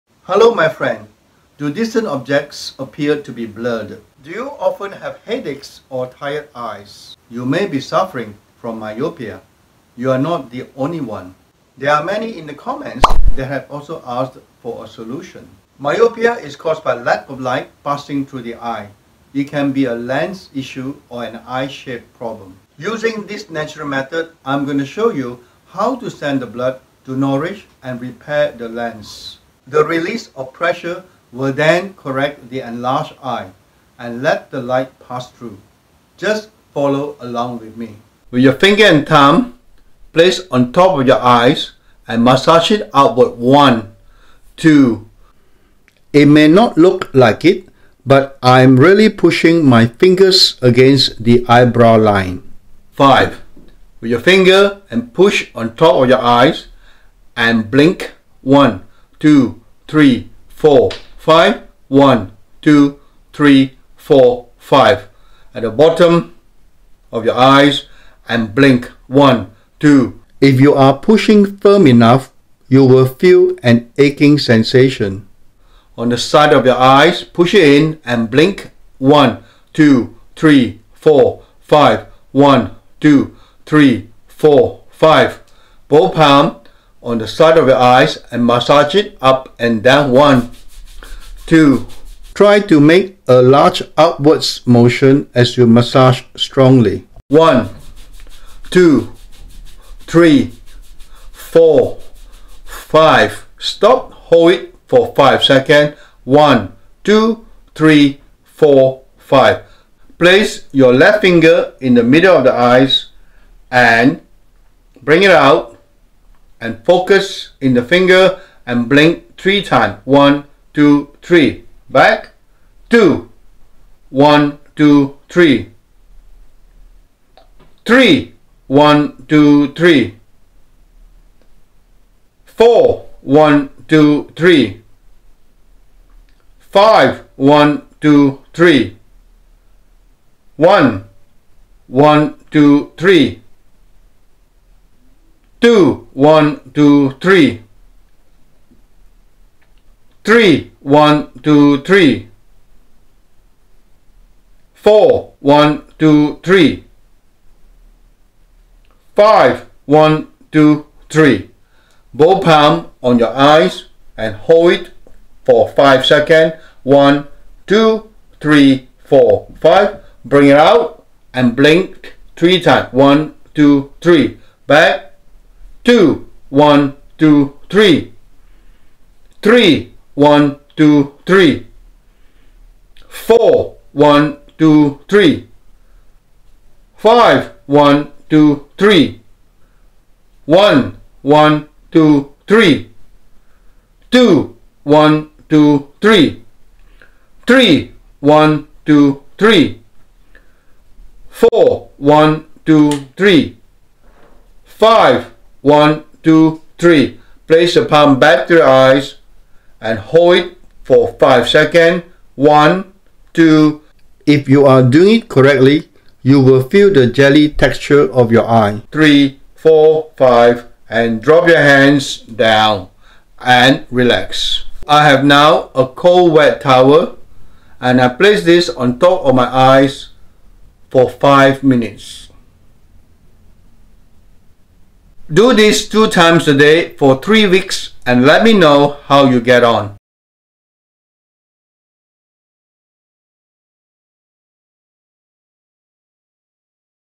How to reduce myopia and improve your eye sight naturally - the natural method
🎁 [FREE CASE STUDY] - How I Get Rid of Chronic Congestion and Breathe Better:

In this video I show you how to reduce your myopia step by step. This is the natural method solution for myopia.

Myopia is caused by the cornea or the lens being too curved this causes short sightedness that’s mean that’s when the eyes lens grow slightly too long stopping the light to the back of the eyes properly. You will have problem seeing distant object.

The video is designed to reduce the curve of the lens by massaging and manipulating the flow of blood into the affected areas to correct the cornea back to it normal size. Letting the light to the back of the eyes so you can be able to see distant object clearly.

You need to follow the exercises step by step and do it two times a day for three weeks.
Do this 2 times a day for 3 weeks.